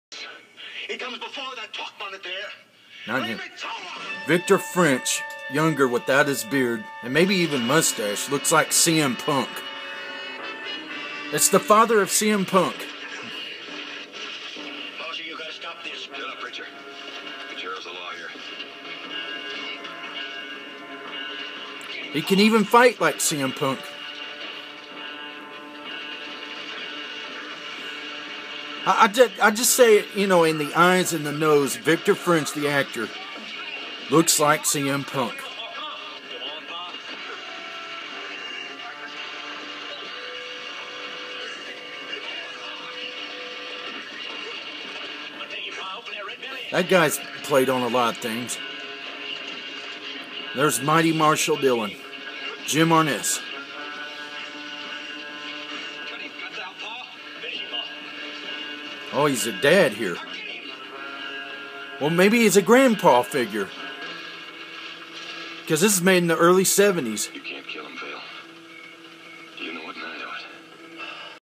Victor French = a OLDER CM Punk?! -The Landaux Show Clip!
(Recorded May 9th 2014) I always thought that Victor French in the early 70s as a guest star on Gun Smoke looks like a older CM PUNK in the eyes lol! It maybe his long lost grand-dad! :-D